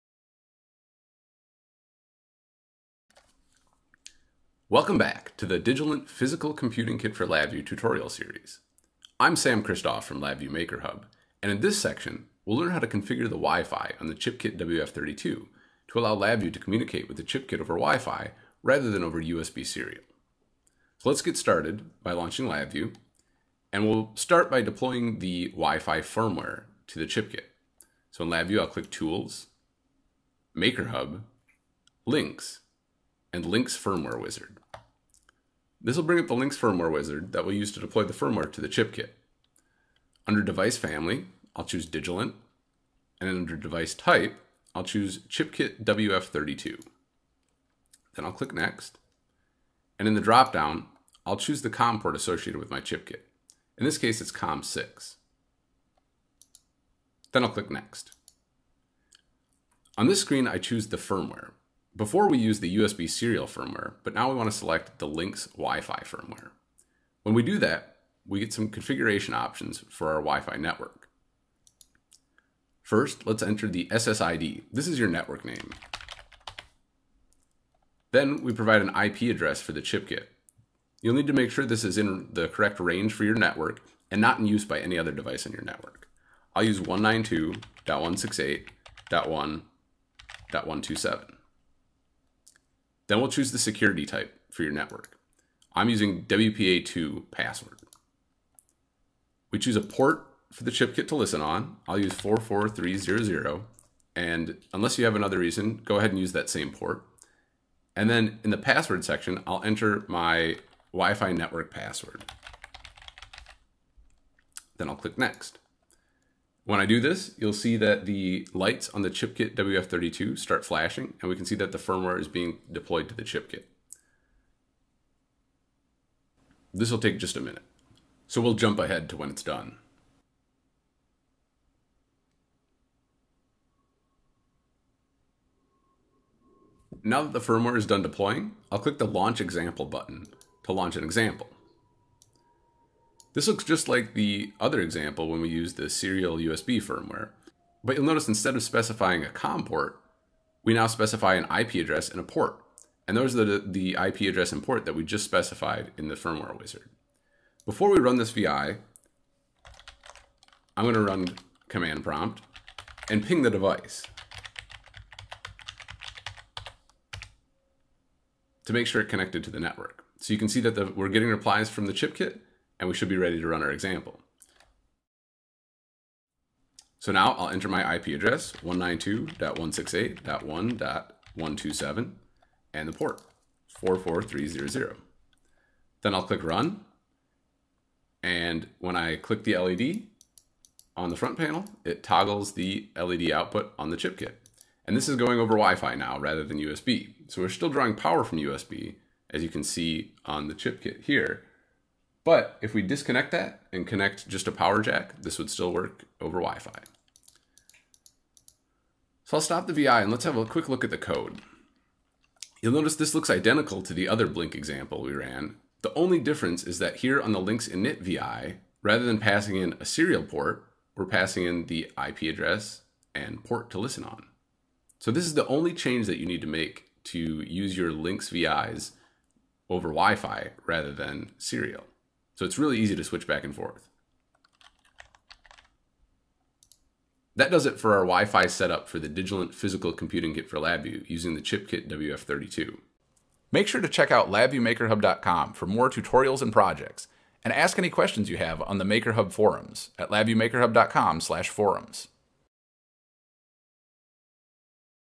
Digilent Physical Computing Kit for LabIVEW - 09 | Wifi Setup
The chipKIT WF32 has an onboard Wifi radio that can be used to connect the WF32 to a Wifi network and control it remotely using LabVIEW. In this tutorial we'll configure the chipKIT WF32's Wifi settings and run a simple Blink example via Wifi.

In this tutorial we'll learn how to download and install LINX from the LabVIEW Tools network. Then we'll setup the chipKIT WF32, load the LINX firmware onto the board and run a simple Blink example.

MakerHub's Digilent Physical Computing Kit for LabVIEW Tutorial Series